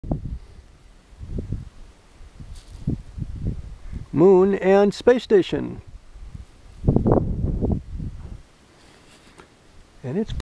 Moon and Spacestation!
Darn it! I should have had my camera running sooner! The spacestation passed right next to the moon!